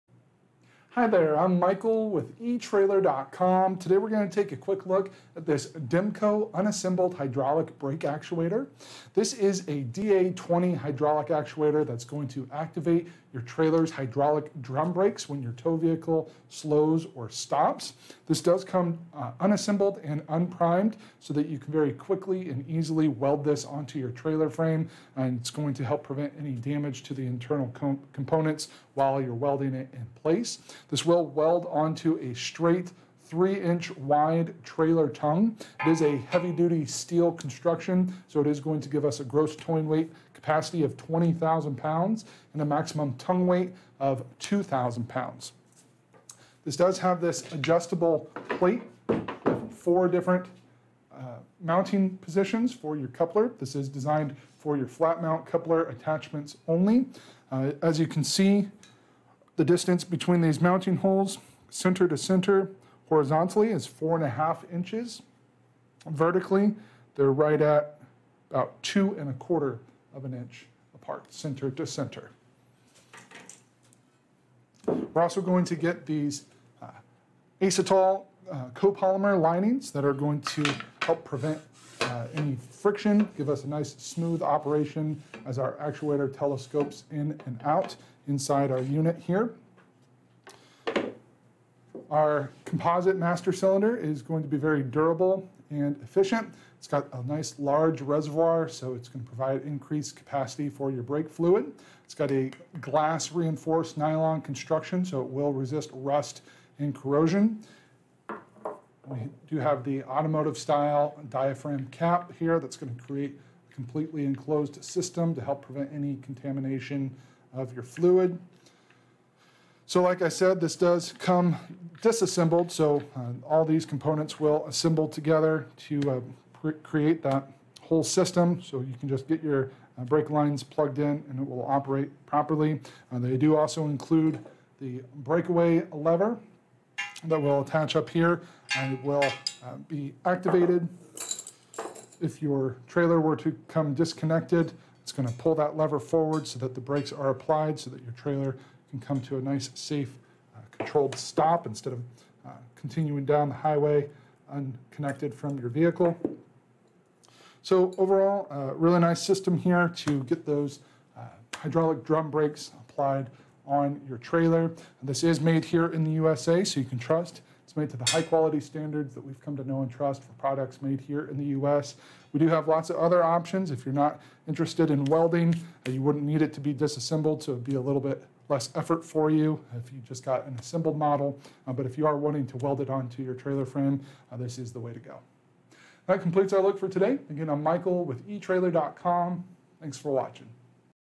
etrailer | Feature Review Demco Unassembled Hydraulic Brake Actuator
Hi there, I'm Michael with etrailer.com. Today, we're gonna take a quick look at this Demco Unassembled Hydraulic Brake Actuator. This is a DA20 hydraulic actuator that's going to activate your trailer's hydraulic drum brakes when your tow vehicle slows or stops. This does come unassembled and unprimed, so that you can very quickly and easily weld this onto your trailer frame, and it's going to help prevent any damage to the internal components while you're welding it in place. This will weld onto a straight three inch wide trailer tongue. This is a heavy-duty steel construction, so it is going to give us a gross towing weight capacity of 20,000 pounds and a maximum tongue weight of 2,000 pounds.

This does have this adjustable plate for different mounting positions for your coupler. This is designed for your flat mount coupler attachments only. As you can see, the distance between these mounting holes, center to center horizontally, is 4 1/2 inches, vertically, they're right at about 2 1/4 of an inch apart, center to center. We're also going to get these acetal copolymer linings that are going to help prevent any friction, give us a nice, smooth operation as our actuator telescopes in and out inside our unit here. Our composite master cylinder is going to be very durable and efficient, it's got a nice, large reservoir, so it's gonna provide increased capacity for your brake fluid.

It's got a glass reinforced nylon construction, so it will resist rust and corrosion. We do have the automotive style diaphragm cap here that's gonna create completely enclosed system to help prevent any contamination of your fluid. So like I said, this does come disassembled, so all these components will assemble together to create that whole system, so you can just get your brake lines plugged in, and it will operate properly. They do also include the breakaway lever that will attach up here and it will be activated, if your trailer we're to come disconnected, it's gonna pull that lever forward, so that the brakes are applied, so that your trailer can come to a nice, safe, controlled stop instead of continuing down the highway unconnected from your vehicle. So overall, really nice system here to get those hydraulic drum brakes applied on your trailer.

This is made here in the USA, so you can trust it's made to the high quality standards that we've come to know and trust for products made here in the US. We do have lots of other options, if you're not interested in welding, you wouldn't need it to be disassembled, so it'd be a little bit less effort for you if you just got an assembled model, but if you are wanting to weld it onto your trailer frame, this is the way to go. That completes our look for today.